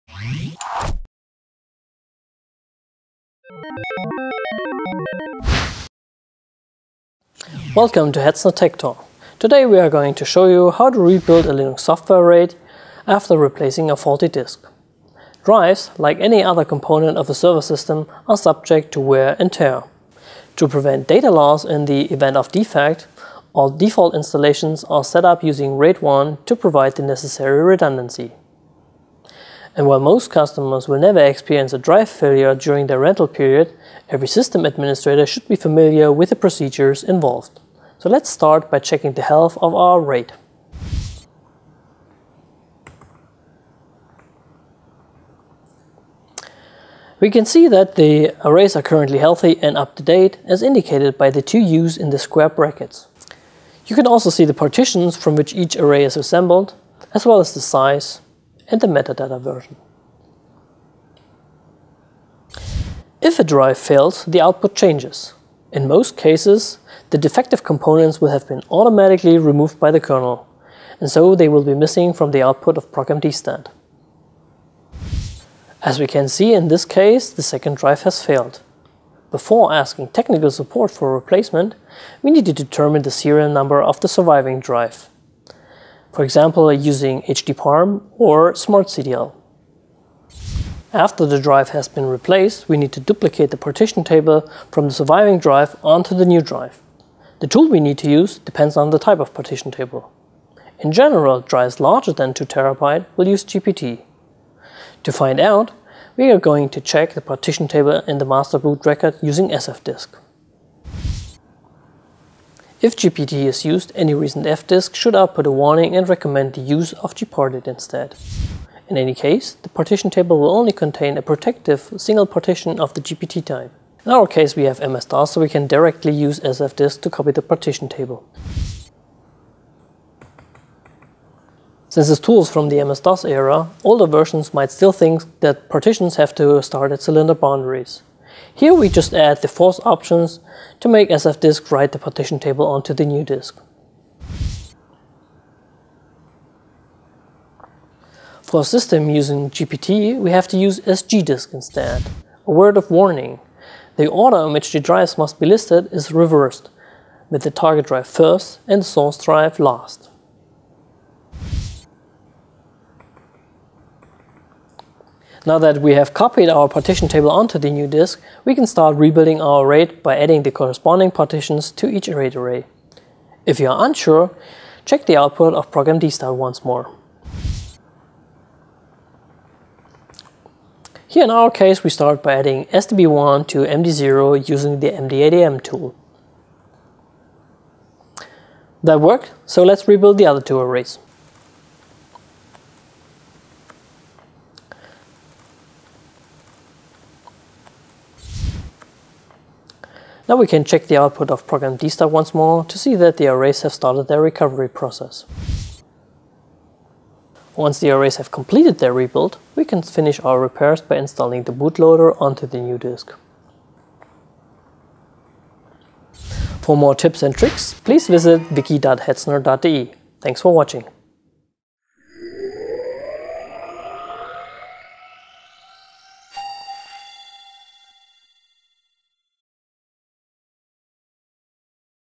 Hetzner TkkTalk - How to rebuild a Linux software RAID
Product Manager Markus Schade describes the necessary steps involved exchanging a defective drive in a Software RAID (mdadm).